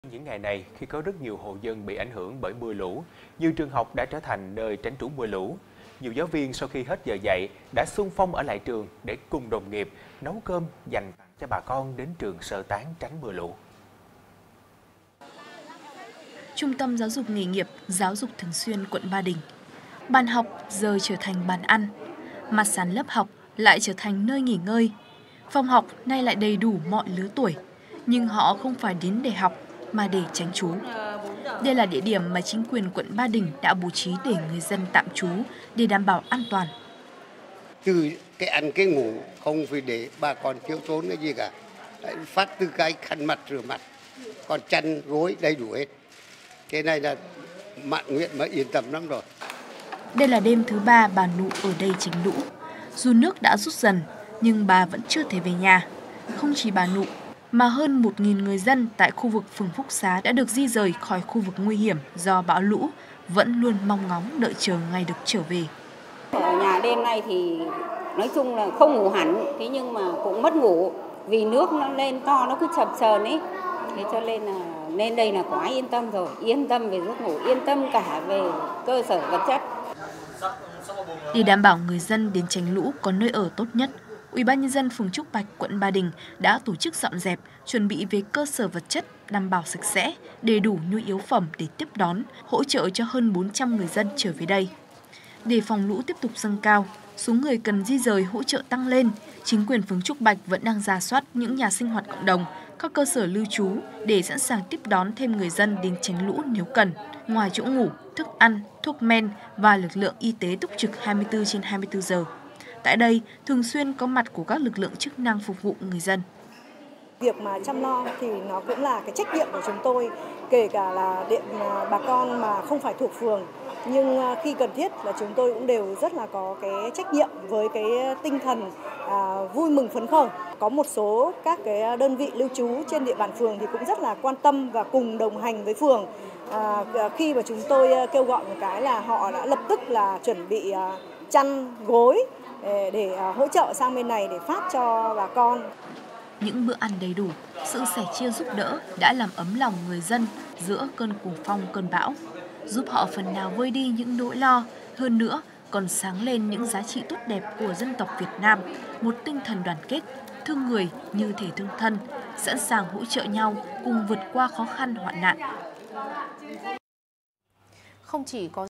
Khi trường học trở thành điểm sơ tán dân: Người dân an tâm tránh lũ | VTC1
VTC1 | Khi trường học trở thành điểm sơ tán dân: Người dân an tâm tránh lũ
Trong những ngày này, khi có rất nhiều hộ dân bị ảnh hưởng bởi mưa lũ, nhiều trường học đã trở thành nơi tránh trú mưa lũ. Nhiều giáo viên sau khi hết giờ dạy đã xung phong ở lại trường để cùng đồng nghiệp nấu cơm dành tặng bà con đến trường sơ tán tránh mưa lũ.